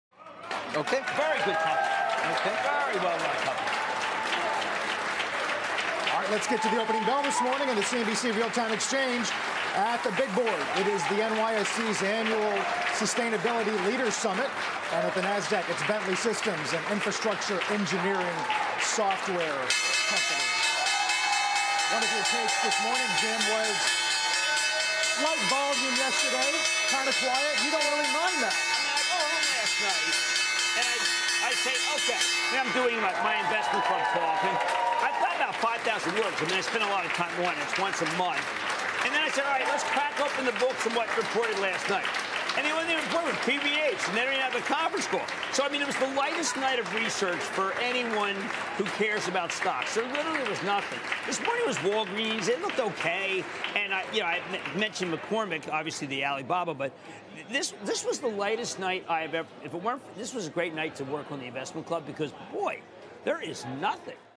Opening Bell: March 28, 2023
Watch as the Opening Bell rings in the new trading session from the New York Stock Exchange and the NASDAQ.